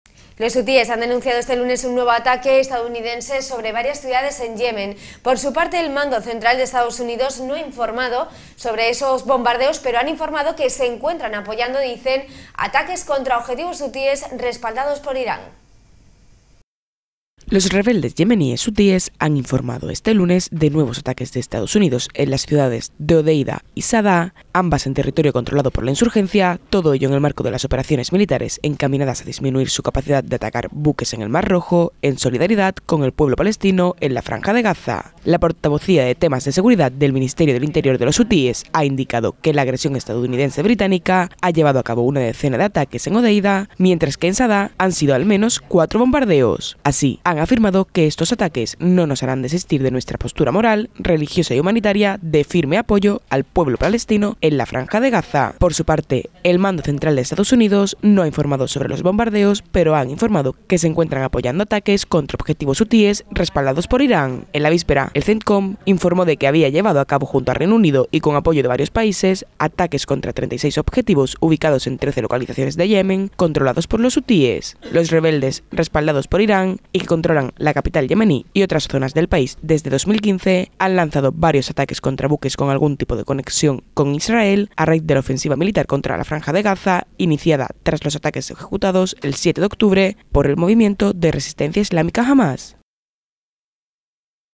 🔴Noticia - Los hutíes denuncian un ataque de EEUU en Yemen
Los hutíes denuncian un nuevo ataque estadounidense sobre varias ciudades en Yemen.Por su parte, el Mando Central de Estados Unidos no ha informado sobre los bombardeos, pero han informado que se encuentran apoyando ataques contra objetivos hutíes respaldados por Irán.
English:

The Houthis denounce a new US attack on several cities in Yemen. For its part, the US Central Command has not reported on the bombings, but they have reported that they are supporting attacks against Houthi targets backed by Iran.

Live streaming